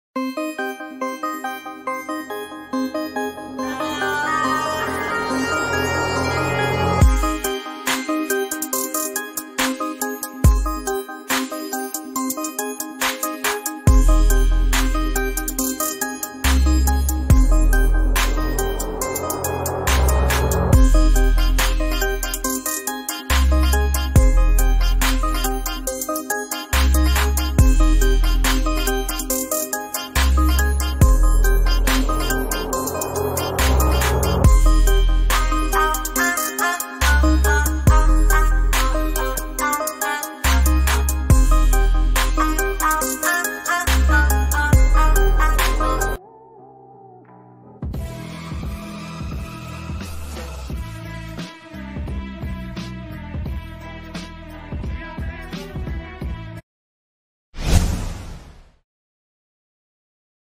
How to download Youtube video in mp3 or mp4.
Hello
DRSN GALAXY

In this video I showed you how to download
 Youtube video in MP3 or MP4.
If this video is useful for you so 👍LIKE this video.